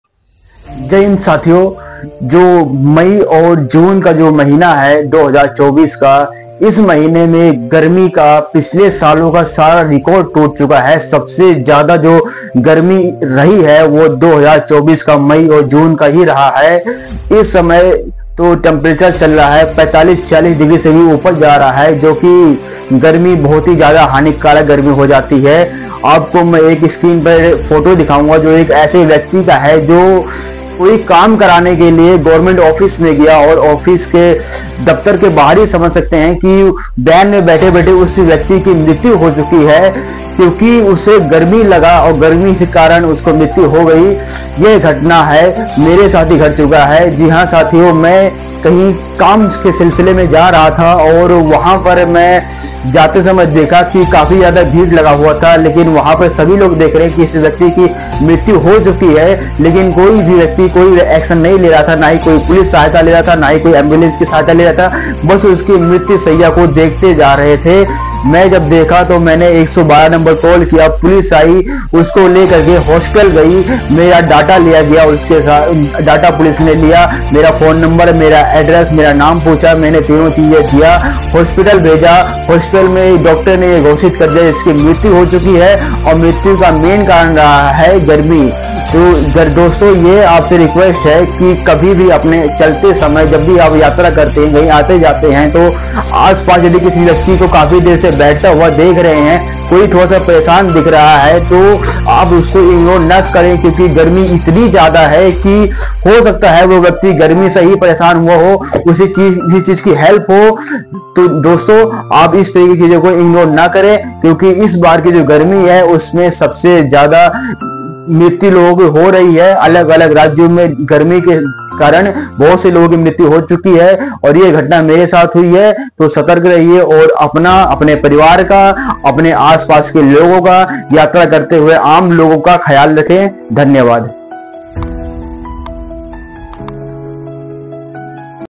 गर्मी गर्मी से हुआ व्यक्ति की मौत मौत heat Man died due to heat रिकॉर्ड तोड़ा इस बार गर्मी
भारत की राजधानी दिल्ली के सिविल लाइन जिले में ट्रांसपोर्ट अथॉरिटी राजपुर रोड पर लाइसेंस के सिलसिले में आए एक व्यक्ति की गाड़ी में बैठे-बैठे हो गई मौत

गर्मी गर्मी से हुआ व्यक्ति की मौत मौत heat Man died due to heat

A person who had come to the Transport Authority Rajpur Road in Civil Lines district of Delhi, the capital of India, for a license, died while sitting in his car.

पिछले सभी सालों के मुकाबले इस बार भारत देश के अंदर सबसे ज्यादा गर्मी देखने को मिला है 2024 के मैं और जून का महीना एक ऐसा महीना बन चुका है जिसमें इस देश की गर्मी 45 से 47 डिग्री तक चली गई है जो कि हमारे देश के लिए बहुत ज्यादा हानिकारक हो सकता है अनेक और राज्यों में आने को लोगों की मृत्यु देखने को मिलती है इस गर्मी के कारण

Compared to all the previous years, this time India has witnessed the highest heat. The month of June 2024 has become such a month in which the heat of this country has reached 45 to 47 degrees, which can be very harmful for our country. In many other states, people are seen dying due to this heat.